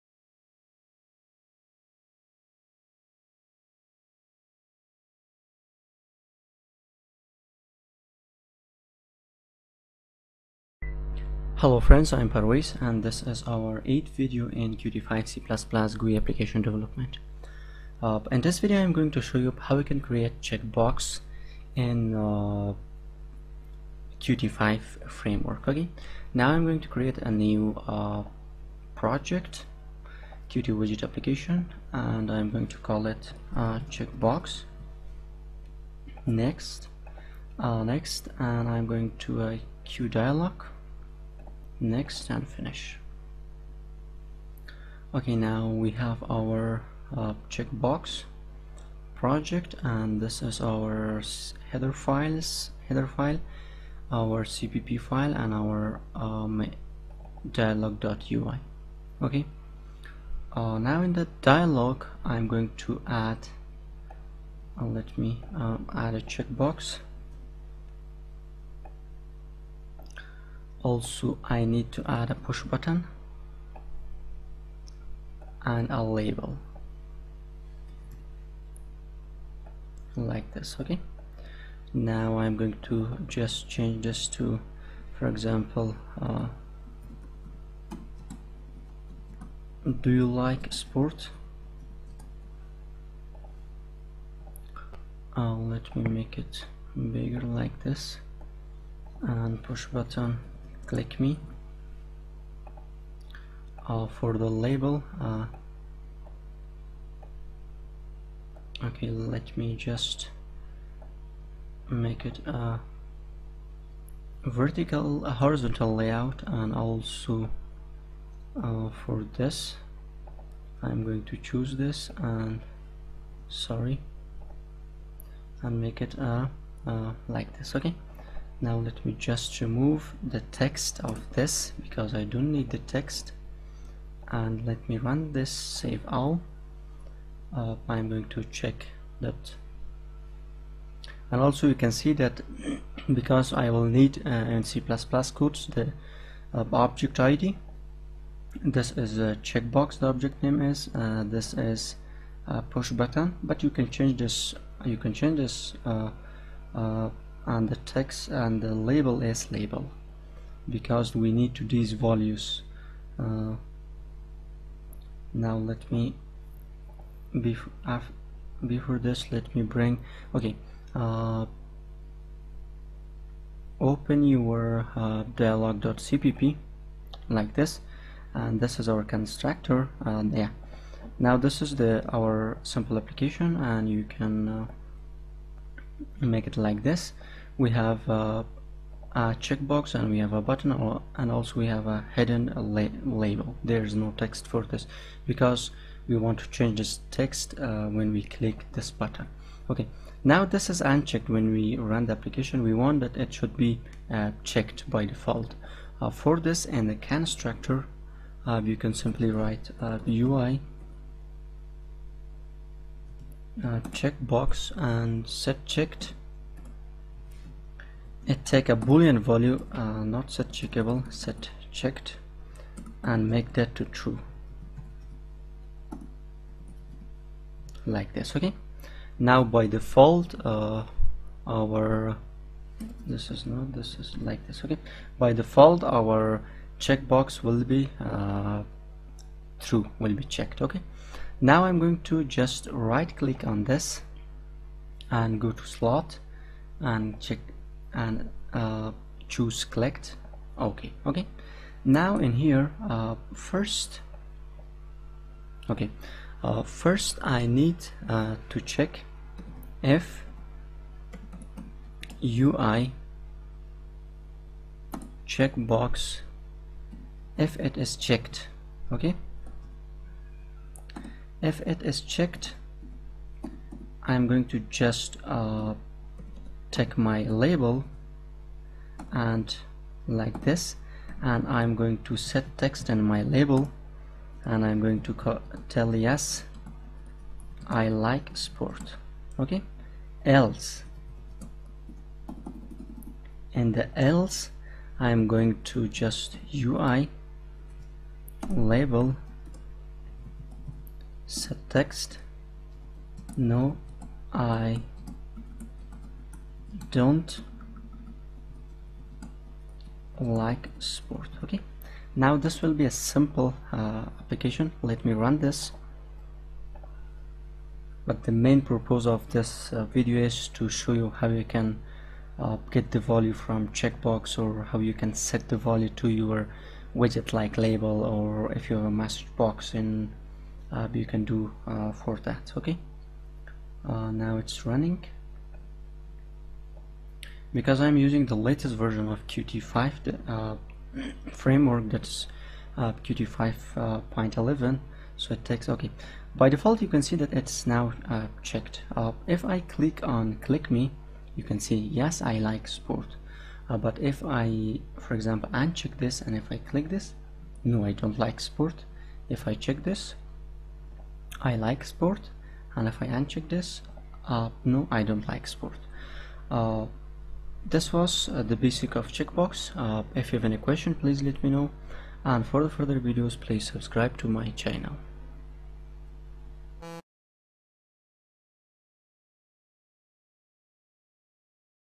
Qt5 C++ Create CheckBox #8
Join Qt6 C++ GUI & Mobile App Development Course in Udemy

In this video iam going to show you how you can create CheckBox in Qt5 C++ and also we will make a simple GUI Application using Qt5 C++ also we will  use Qt5 Signal And Slot with CheckBox